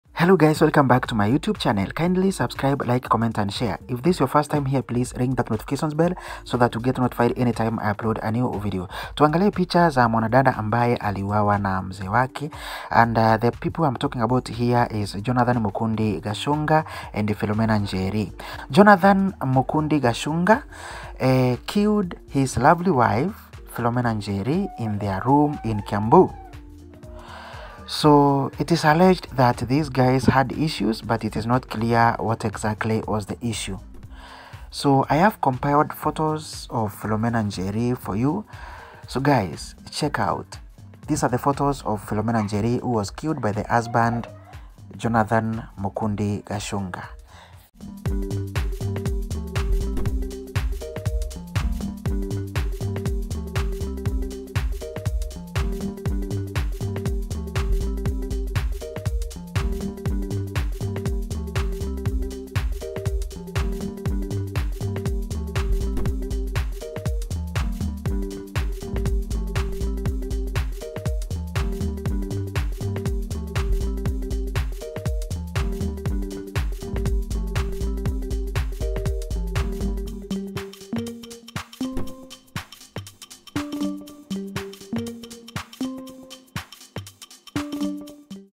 PHOTOS OF PHILOMENA NJERI WHO WAS K#ILLED BY THE HUSBAND IN KIAMBU, JONATHAN MUKUNDI GACHUNGA
Here are the photos of Philomena Njeri who was killed by her husband Jonathan Mukundi Gachunga in Kiambu.